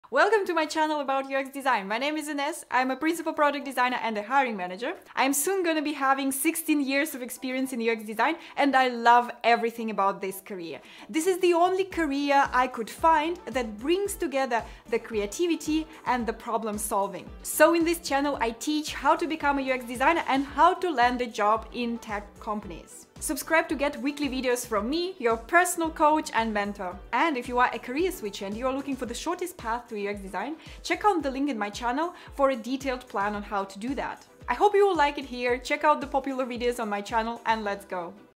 [Trailer] Learn UX design with me and get a job in tech
My name is Inès. I am a principal product designer and hiring manager with 15+ years of experience.